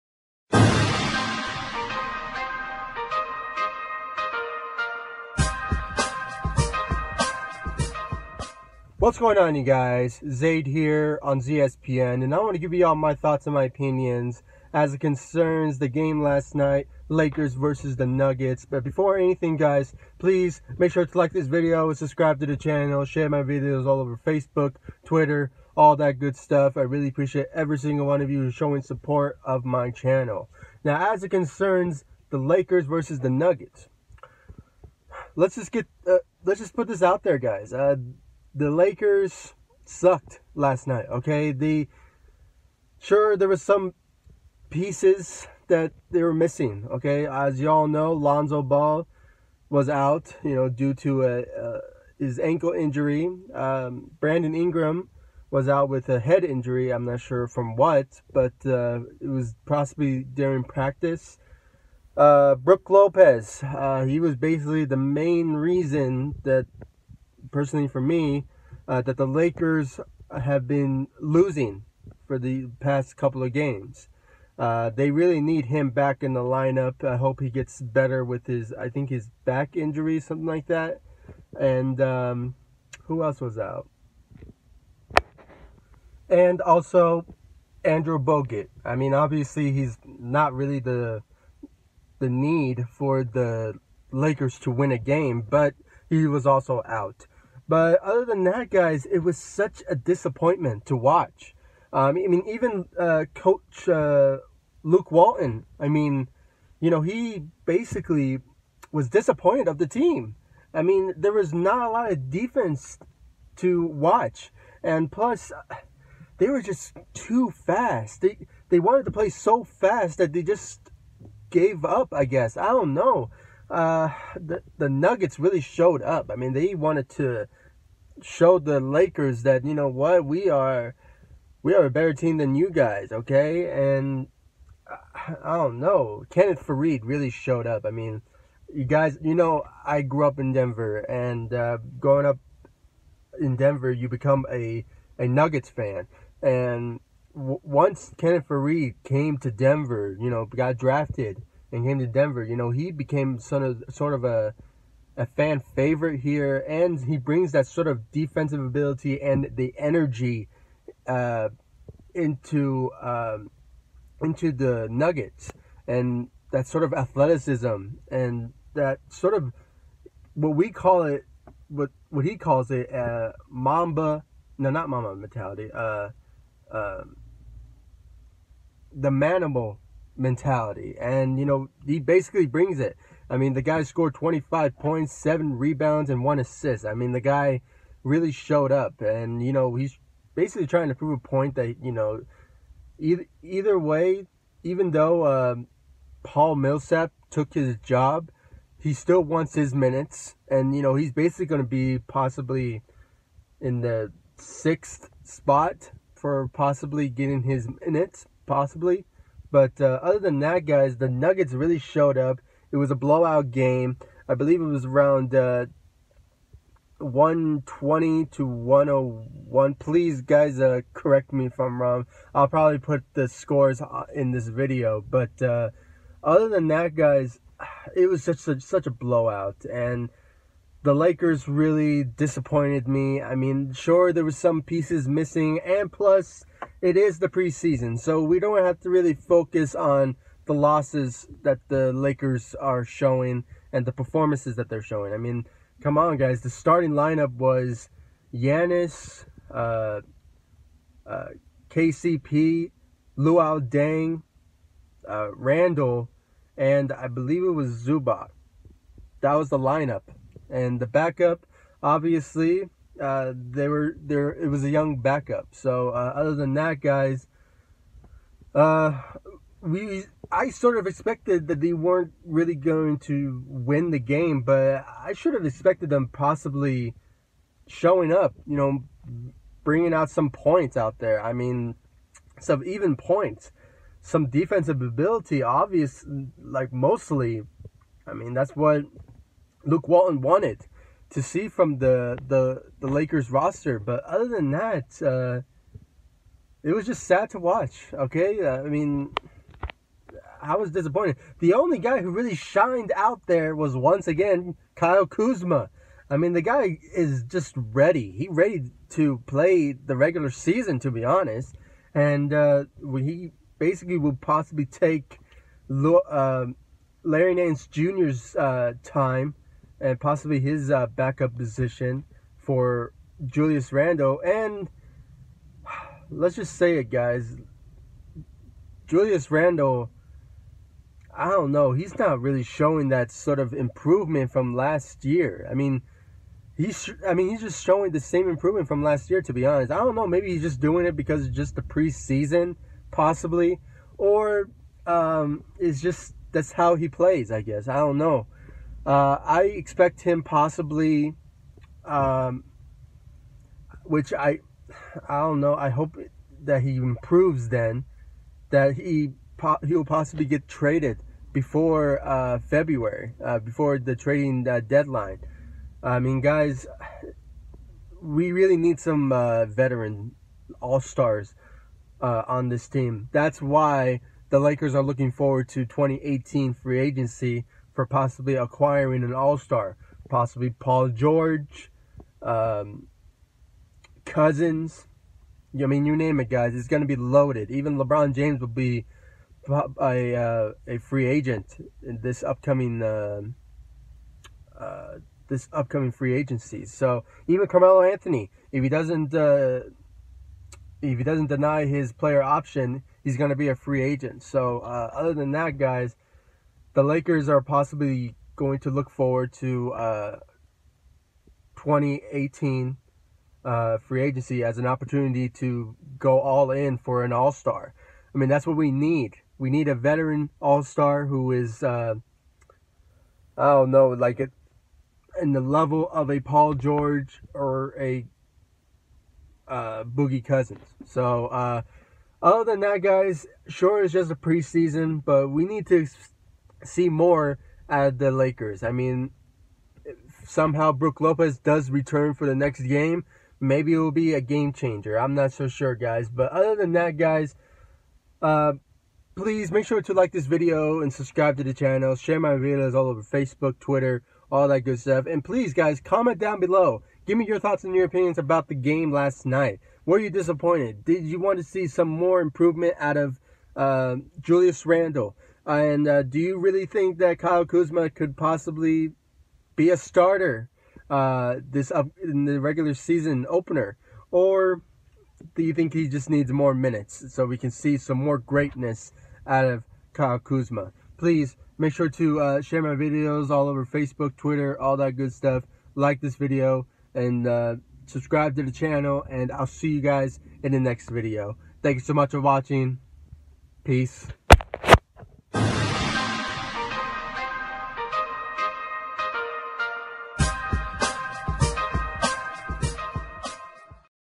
My Thoughts on Lakers loss against the Denver Nuggets Preseason 3 2017
Ball, Ingram, Lopez, and Bogut all out against the Nuggets

Kyle Kuzma 21 Points, 7-9 FG, 2 Rebounds

Nuggets Kenneth Faried 25 Points, 7 Rebounds, 1 assist

PEACE ✌🏽✌🏽✌🏽✌🏽

GO LAKERS!